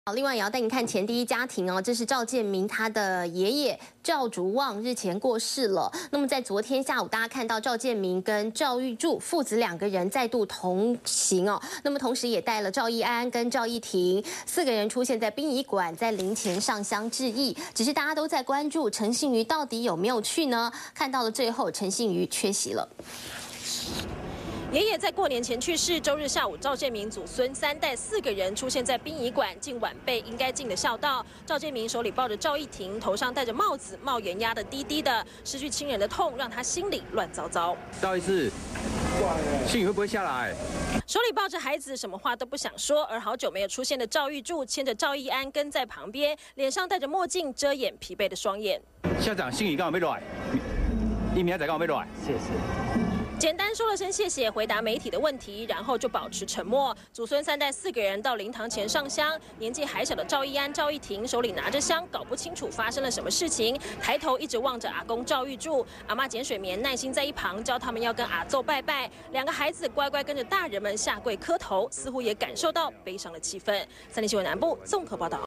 081221 趙建銘 爺去世
趙建銘 爺去世本段是三立還算OK
反觀比較一下其他某幾台 記者包圍問:感覺怎樣??
不回應被批傲慢!